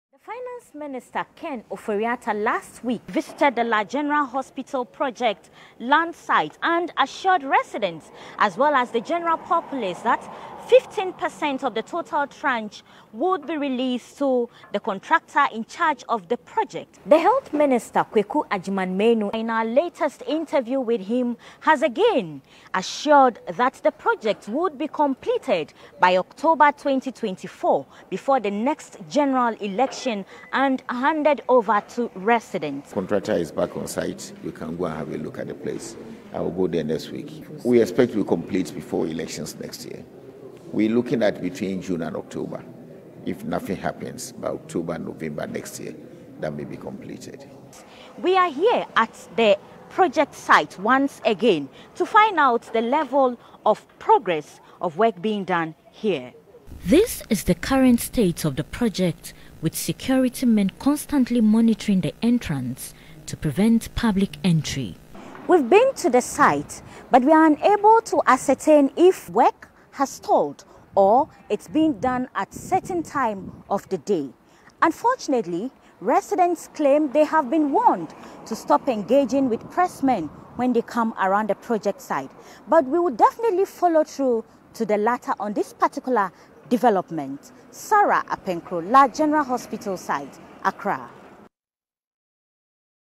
La General Hospital: Project to be completed by October 2024 - Health Minister
Health minister Kwaku Agyeman Manu says the La General Hospital will be completed by October next year, exactly ten months.

This will be the minister's latest assurance to the people of La as work is expected to be progressing at the project site.

Sarah Apenkroh reports.